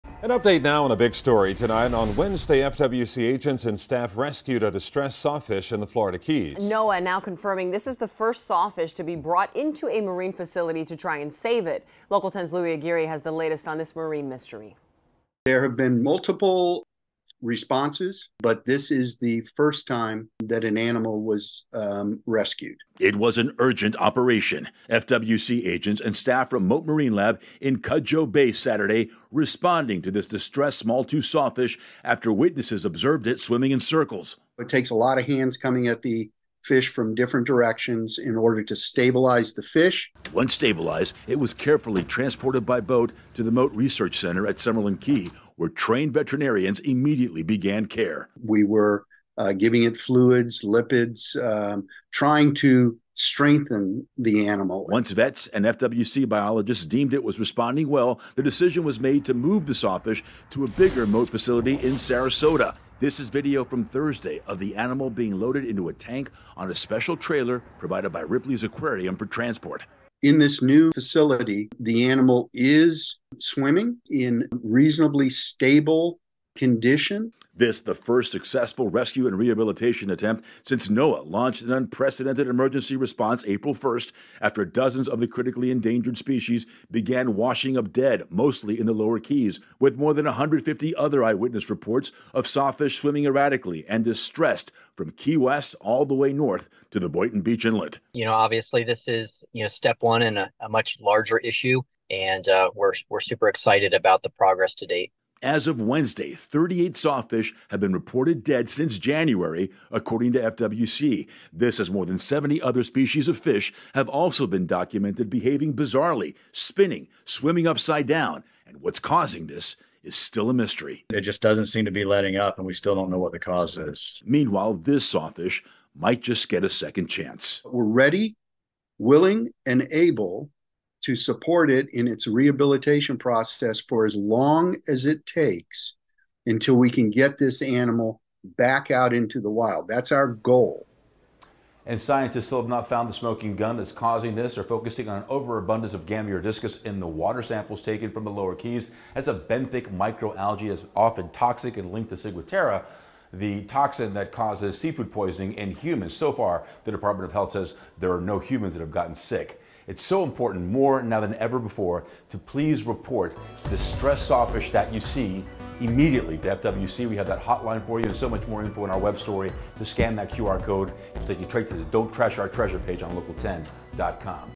Distressed sawfish brought to marine facility for treatment
On Saturday, FWC agents were joined by staff from MOTE Marine Lab to evaluate a distressed smalltooth sawfish swimming in circles in Cudjoe Bay.